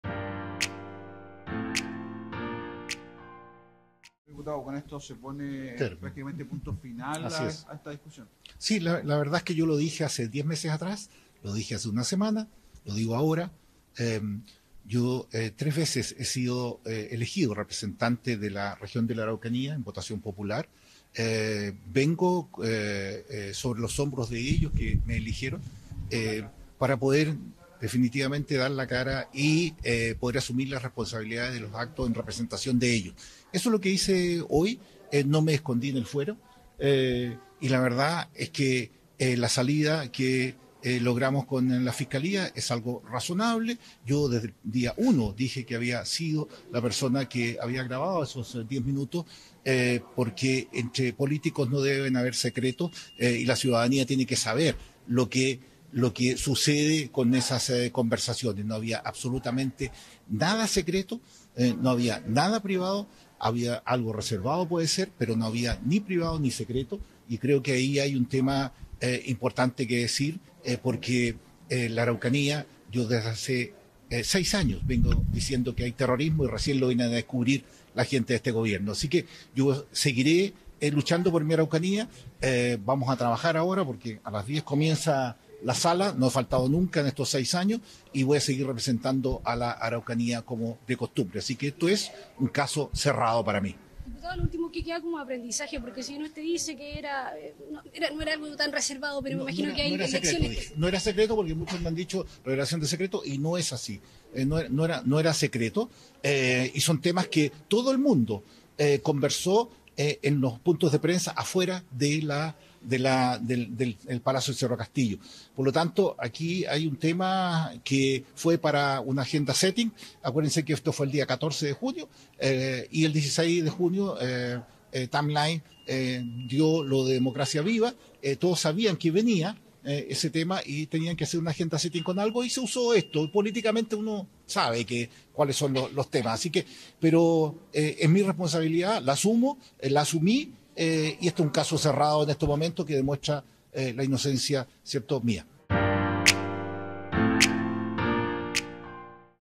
Diputado Mellado acuerda salida alternativa tras filtrar audio de reunión privada con Boric
El diputado de Renovación Nacional, Miguel Mellado, fue formalizado por la filtración del audio de un reunión de carácter privado que sostuvo con el Presidente Gabriel Boric.